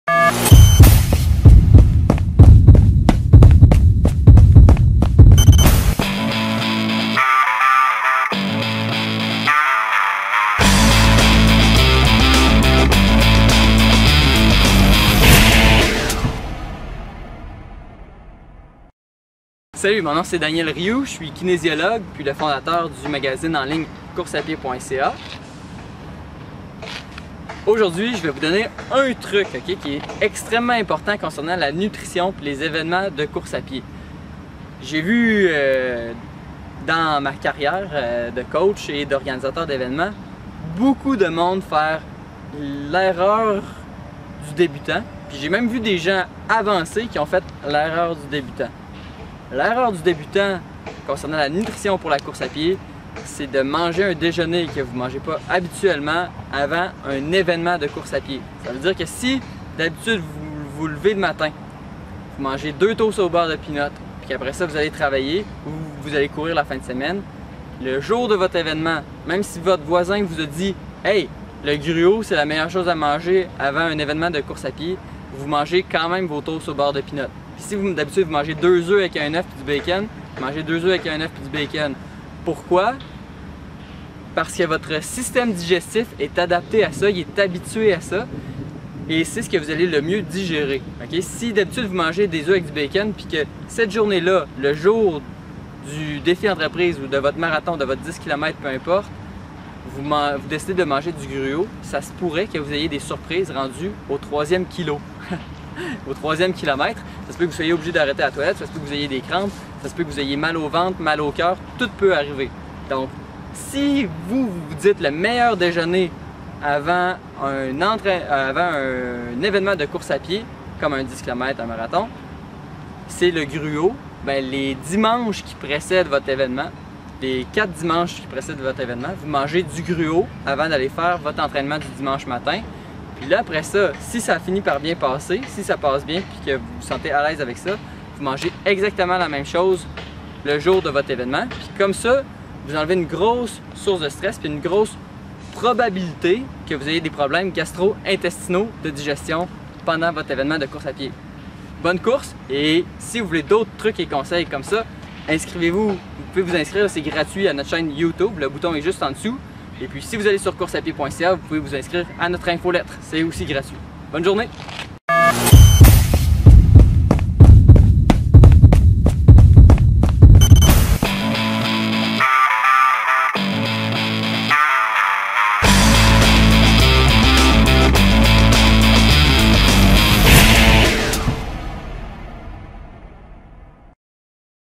Trucs et astuces : Quoi manger le matin d’une compétition
Le matin d'une course, êtes-vous du genre à faire des changements dans vos habitudes ? Ce n'est pas une bonne idée. Voyez ce que Daniel Riou a à dire dans l'astuce du jour.